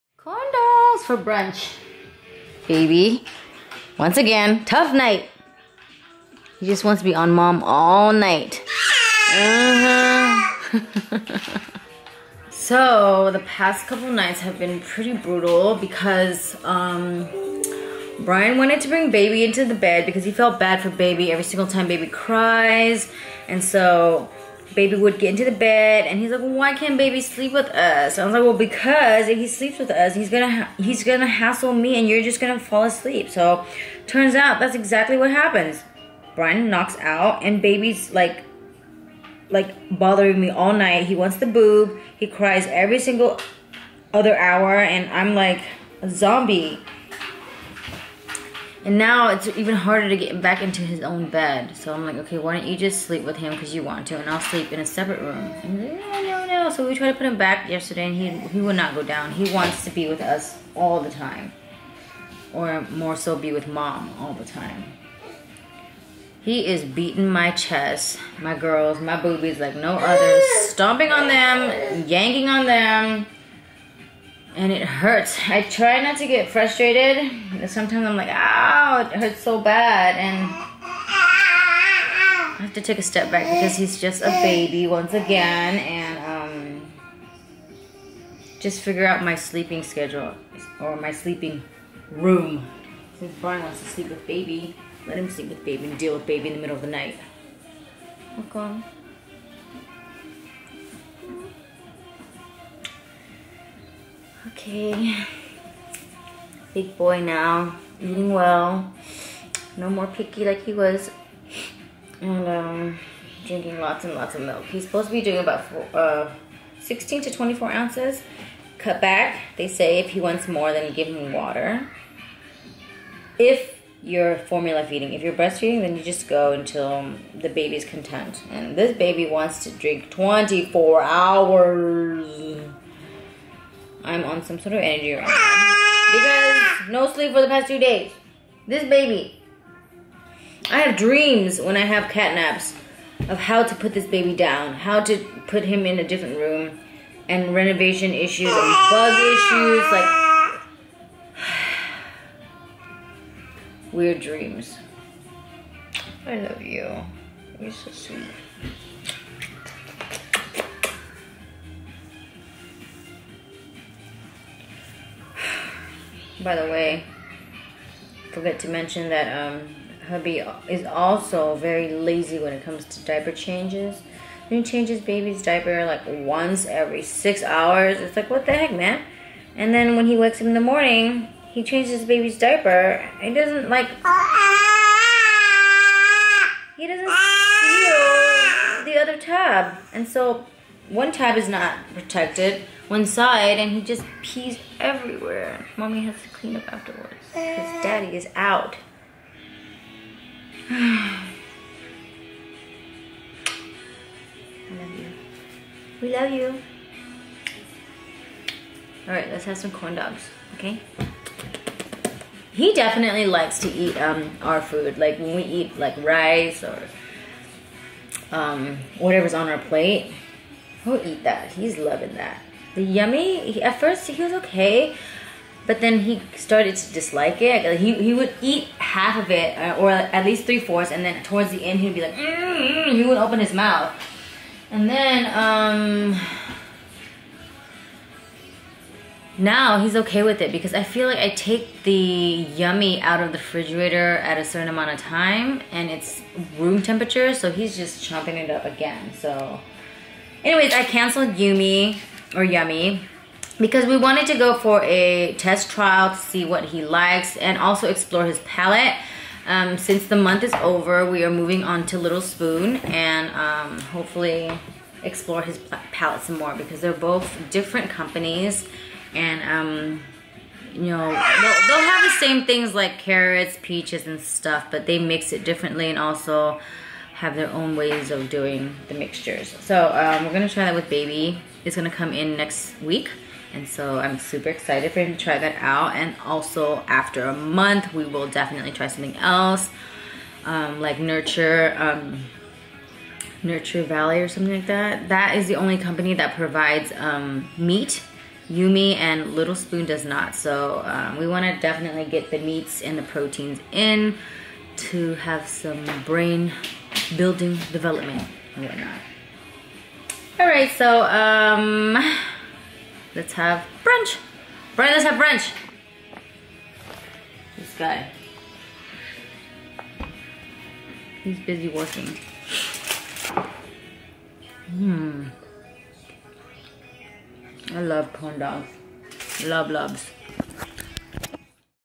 Hubby Messing Up Baby Sleeping Schedule!!!!! | Minnie's VLOG 1125
Hubby is totally messing up baby's sleeping schedule! All my hard work has gone out the door because hubby was needy and wanted to co sleep with baby one night. Now I gotta start all over again and get back on track. It's for the better of his health. Keep order!